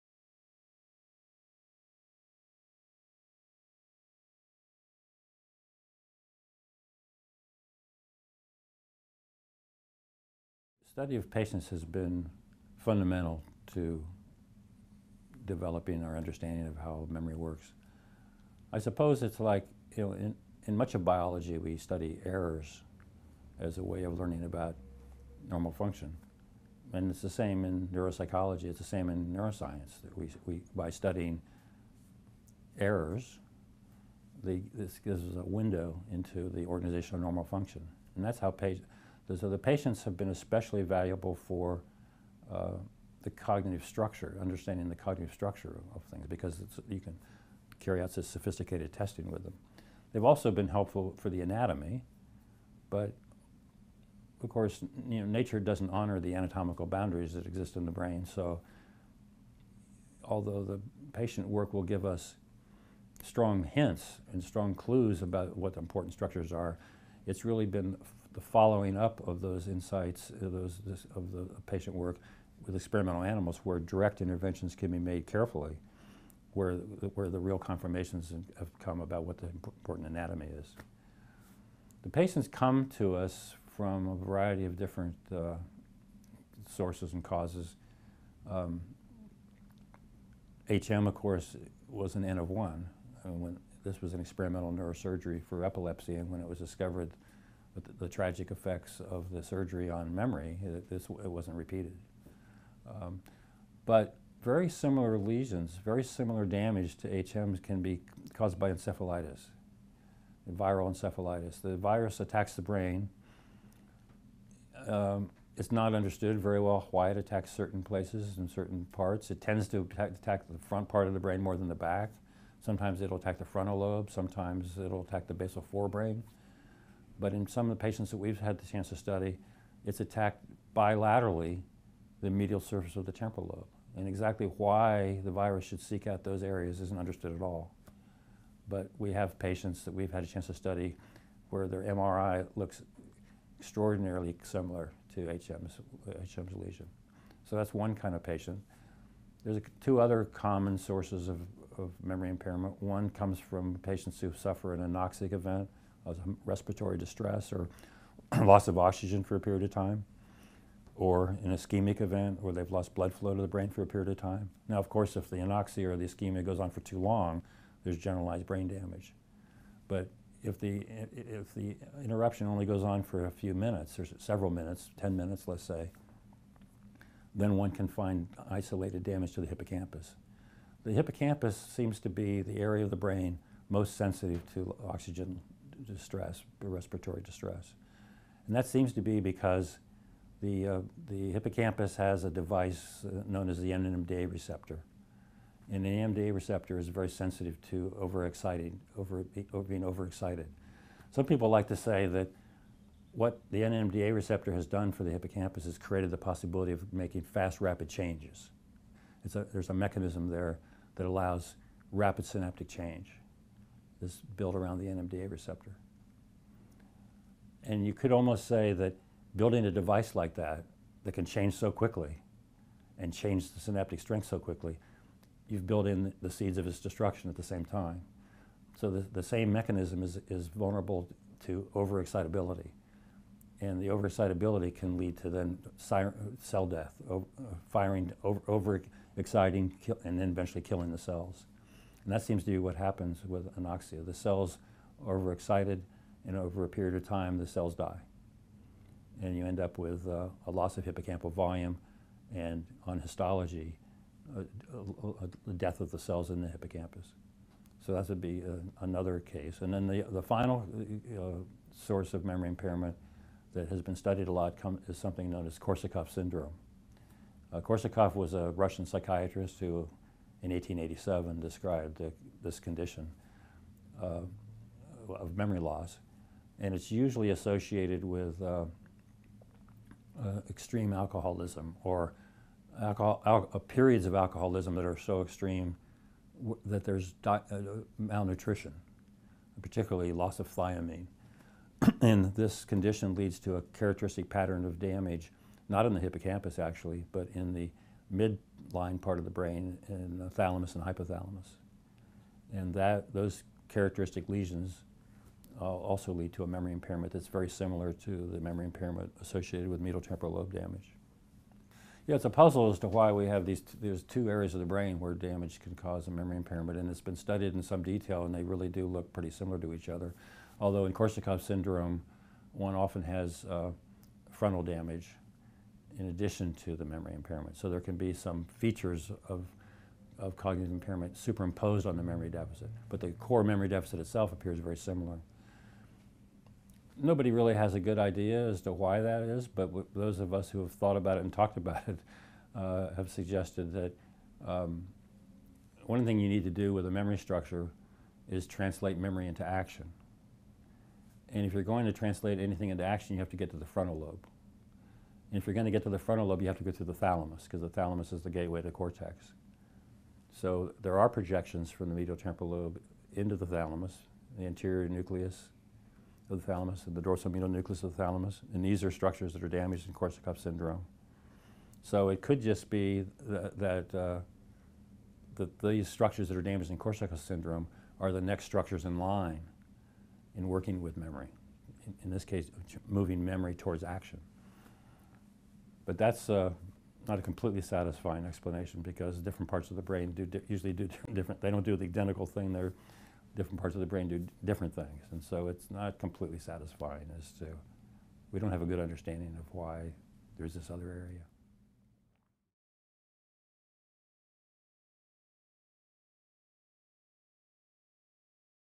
larry squire - the role of patients in memory research
Some of the most important distinctions among memory systems in humans have been derived from the careful analysis of behavior of amnestic patients.  The data clearly shows that working memory and long-term memory are two distinct mechanisms that the brain uses to preserve information after a stimulus has come and gone.  Patient data has also allowed to delineate the difference between multiple long-term memory systems - most importantly a declarative memory system which is largely responsible for conscious memory, and a set of skill-based memory systems that are largely unaffected in amnesia.  Controlled animal studies have helped to pinpoint the exact mechanisms underlying these memory systems.